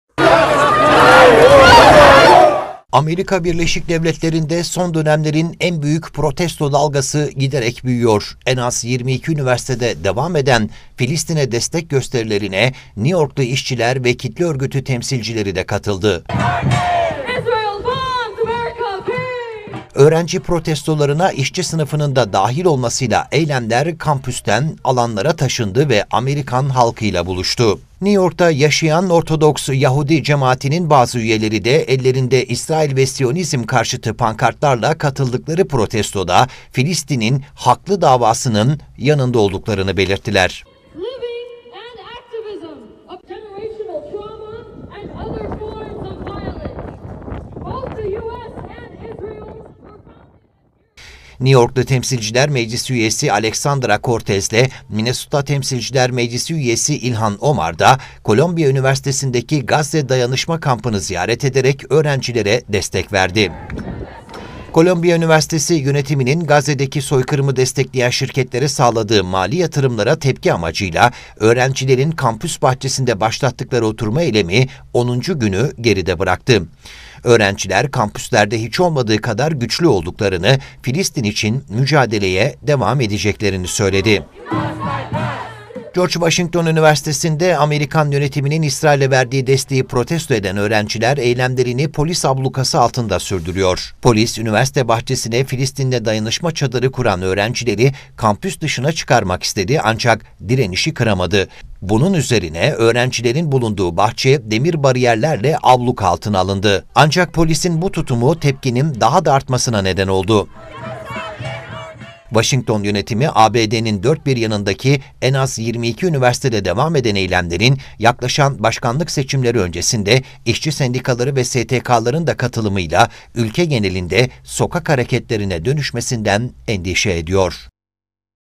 ABD yanıyor! Filistin destekçileri sahaya indi!
ABD’de üniversitelerde başlayan İsrail protestoları, sokağa taştı. Protesto dalgasına işçi sınıfı da dahil oldu. Öğrenciler, eğitimciler, işçiler ve aktivistler, İsrail’in Gazze’deki soykırımını protesto ediyor.

AE-424